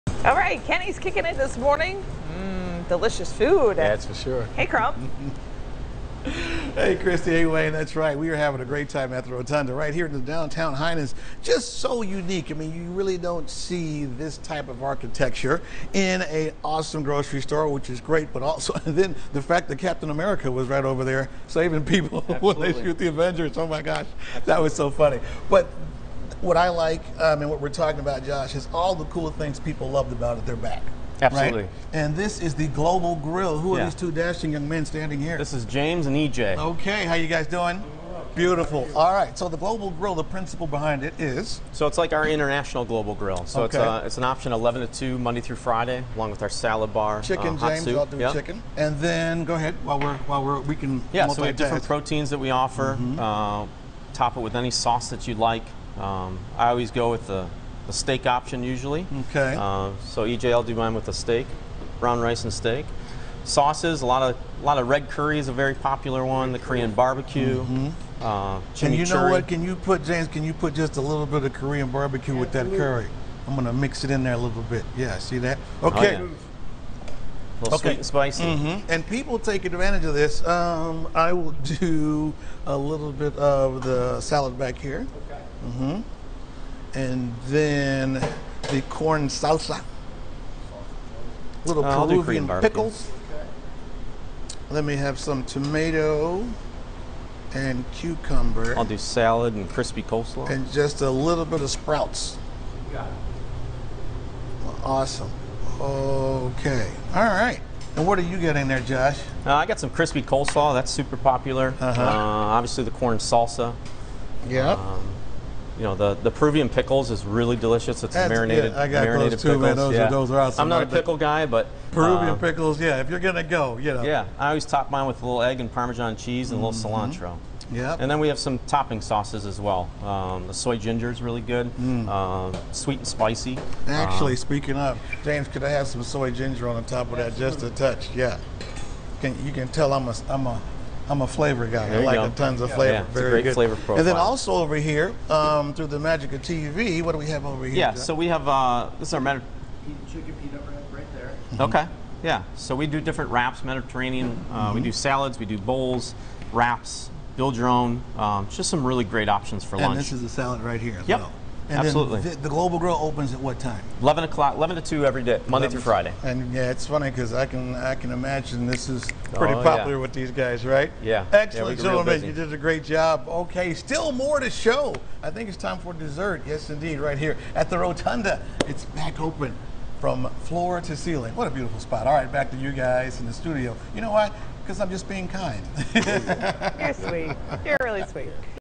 A new spin on grocery shopping: Check out Heinen's Rotunda in downtown Cleveland
Heinen's is thrilled to have its downtown store fully re-opened after the pandemic and Fox 8's Kenny Crumpton ate his way through the beautiful rotunda reminding everyone of  everything the unique grocery store has to offer